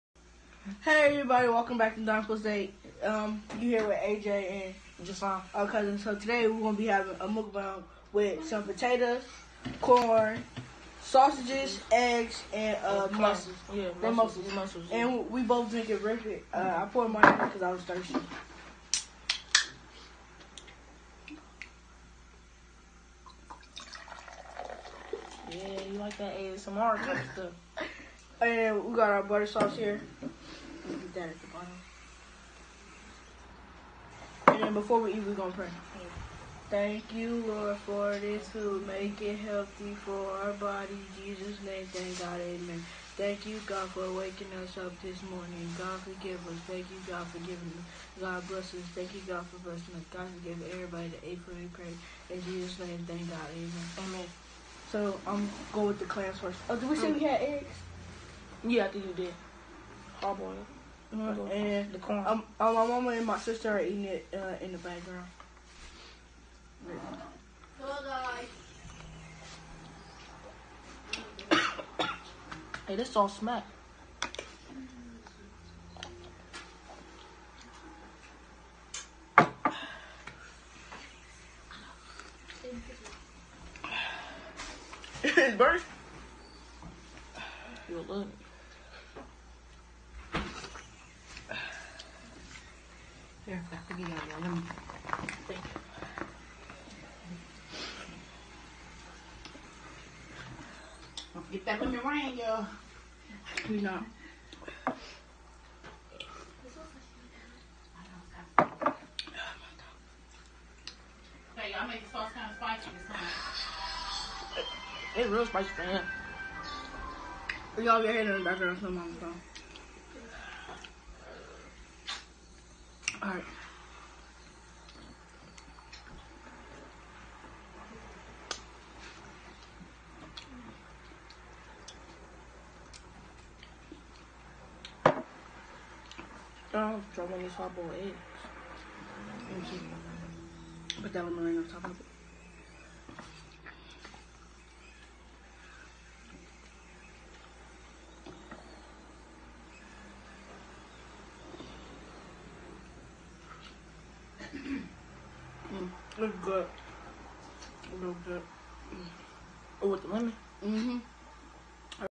Seafood Mukbang (Half Vid)
Sorry guys we cant seem to upload other half but enjoy😇😊❤ Special guest my nephew Jahson😊